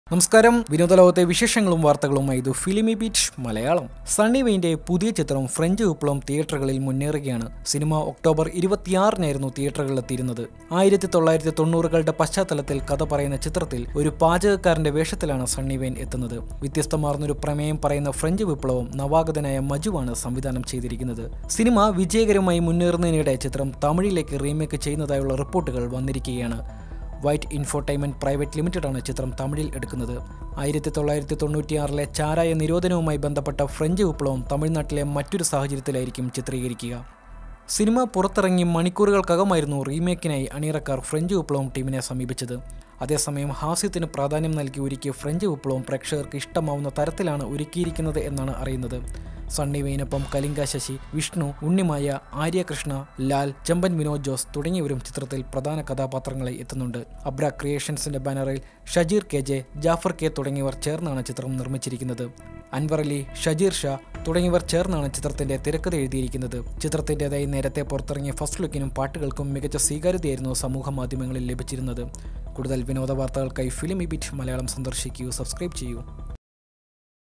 ചിത്രം തമിഴിലേക്ക് റീമേക്ക് ചെയ്യുന്നു | FilmiBeat Malayalam
ഫ്രഞ്ച് വിപ്ലവം വിജയകരമായി മുന്നേറുന്നതിനിടെ ചിത്രം തമിഴിലേക്ക് റീമേക്ക് ചെയ്യുന്നതായുളള റിപ്പോര്‍ട്ടുകള്‍ വന്നിരിക്കുകയാണ്. വൈറ്റ് ഇന്‍ഫോടെയ്ന്‍മെന്റ് പ്രൈവറ്റ് ലിമിറ്റഡാണ് ചിത്രം തമിഴില്‍ എടുക്കുന്നത്. 1996ലെ ചാരായ നിരോധനവുമായി ബന്ധപ്പെട്ട ഫ്രഞ്ച് വിപ്ലവം തമിഴ്‌നാട്ടിലെ മറ്റൊരു സാഹചര്യത്തിലായിരിക്കും ചിത്രീകരിക്കുക.

malayalam movie french viplavam is going to remake in Tamil
Filmibeat Malayalam